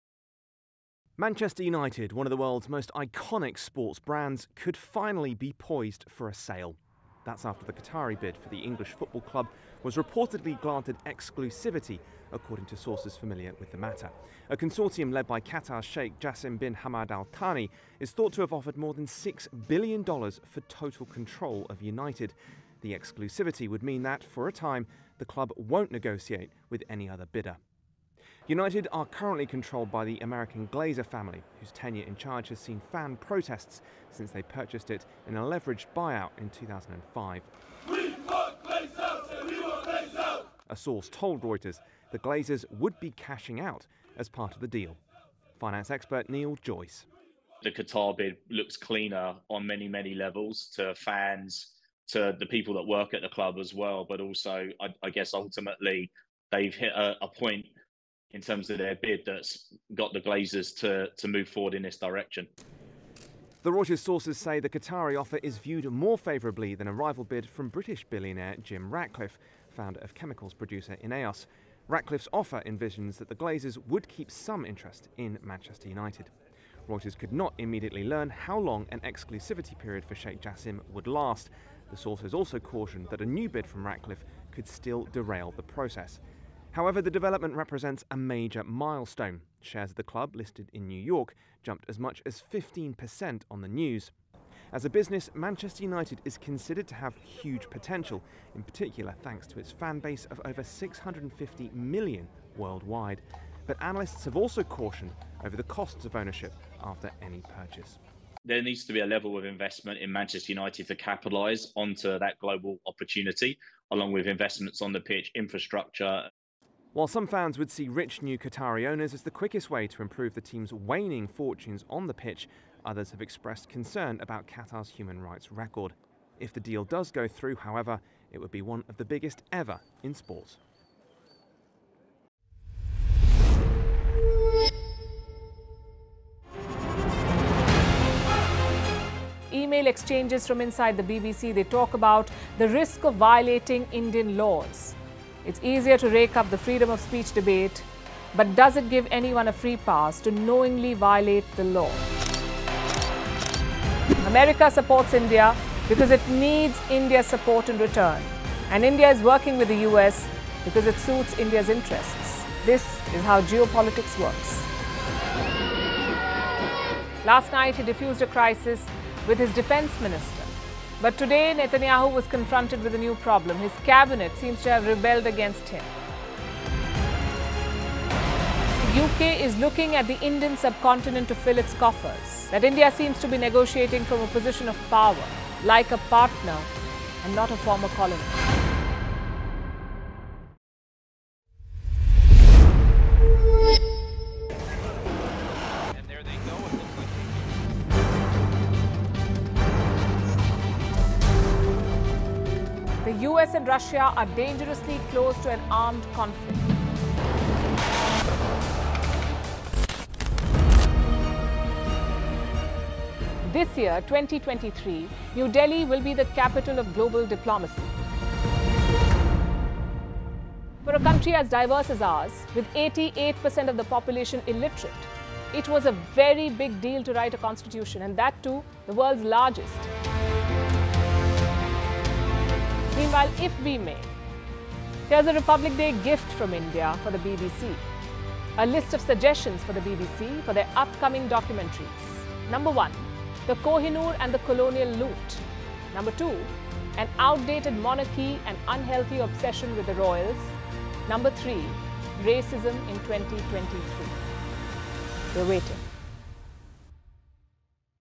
Manchester United Weighing $6 Billion Qatari Sale: Report
English football club Manchester United is negotiating granting exclusivity to the consortium led by Qatar's Sheikh Jassim bin Hamad al-Thani in talks to sell itself for more than $6 billion, people familiar with the matter on June 15.